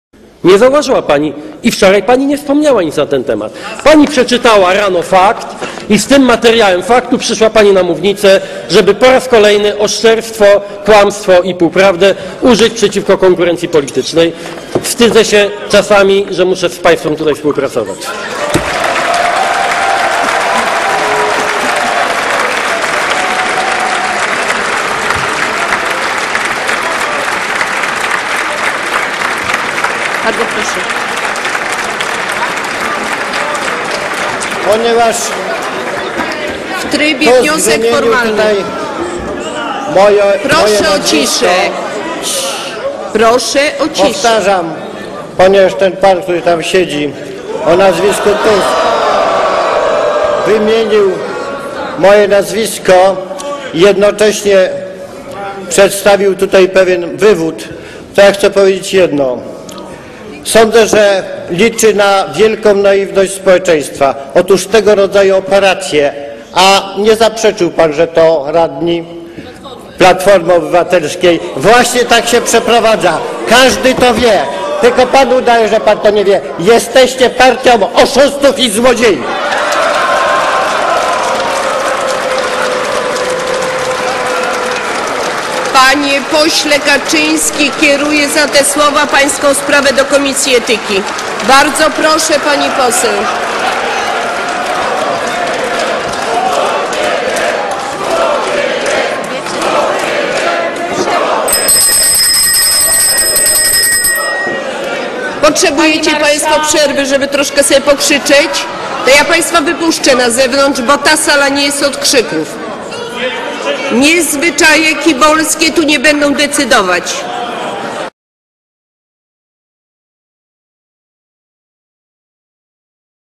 Kaczyński - Jesteście partią oszustów i złodziei
Kliknij DZWONEK 🔔 aby być na bieżąco!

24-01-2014 - Ostro w Sejmie. Premier Donald Tusk zarzucił politykom PiS kłamstwo ws. sprzedaży mysłowickiego szpitala. W odpowiedzi prezes PiS Jarosław Kaczyński nazwał z mównicy sejmowej PO partią "złodziei i oszustów". W reakcji marszałek Sejmu Ewa Kopacz zapowiedziała wniosek do komisji etyki dodając, że "zwyczaje kibolskie tu nie będą decydować".